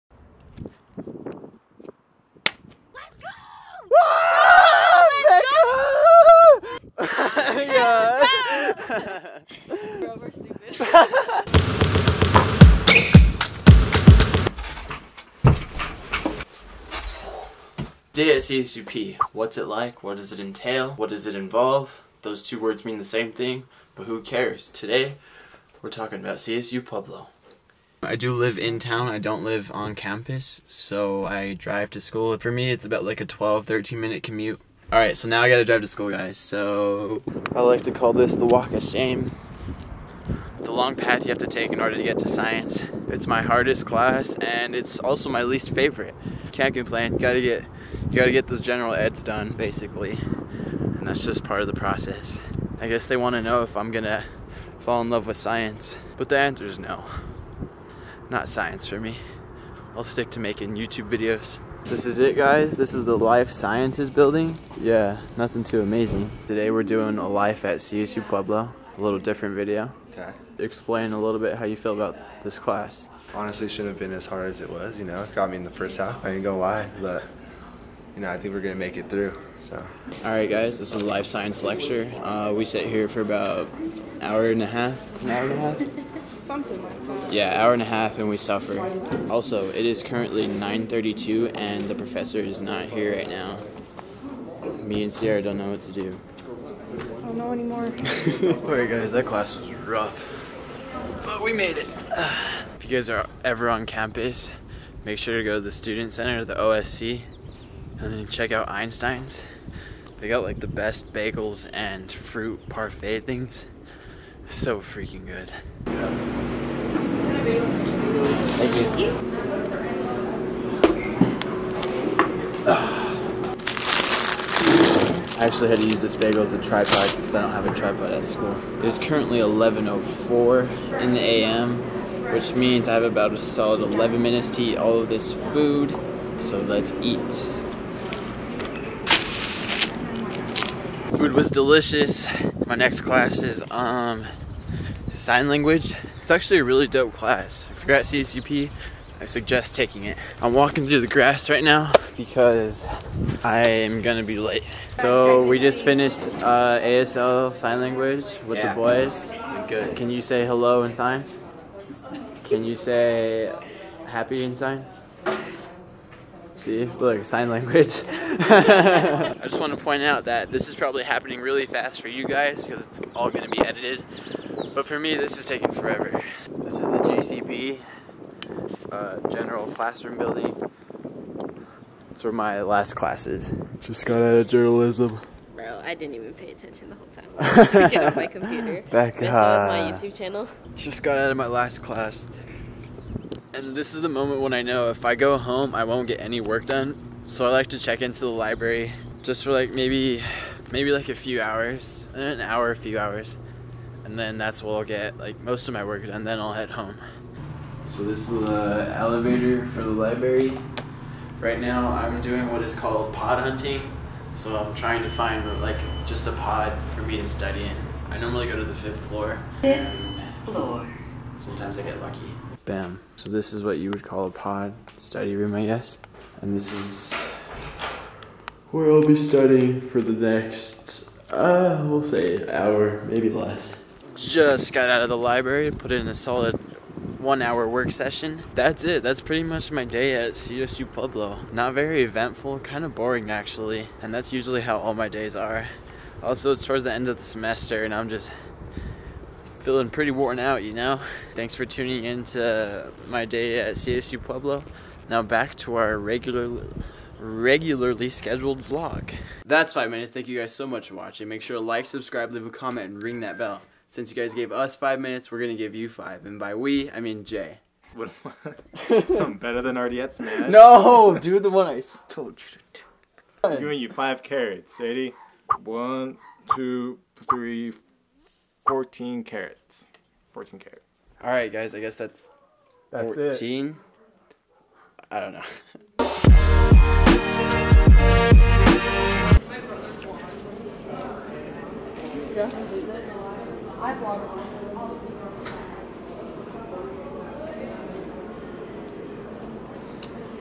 DAY IN THE LIFE of A CSU-P Freshmen
Welcome to college!

I just go through my day, which is kind of boring, at CSU-P

NEW VLOGS EVERY MONDAY WEDNESDAY and  FRIDAY

SUPPORT THESE PEOPLE